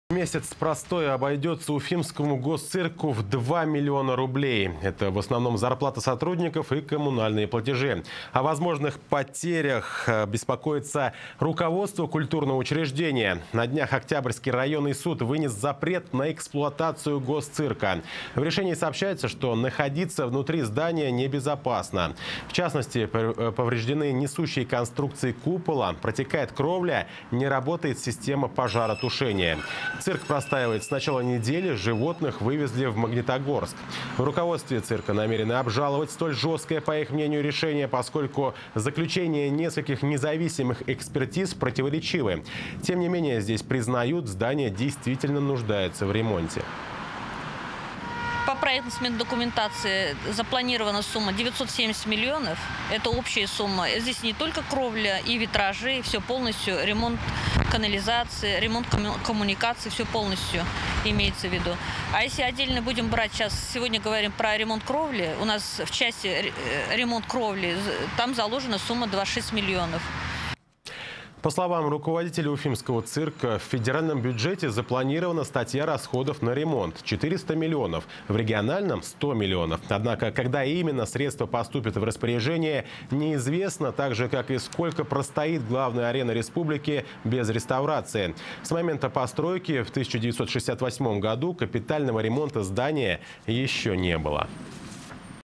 Месяц простоя обойдётся Уфимскому цирку в 2 млн. руб.
Месяц простоя обойдется Уфимскому госцирку в два миллиона рублей: это в основном зарплата сотрудников и коммунальные платежи.

Комментарий: Гюзель Туктарова, директор уфимского госцирка.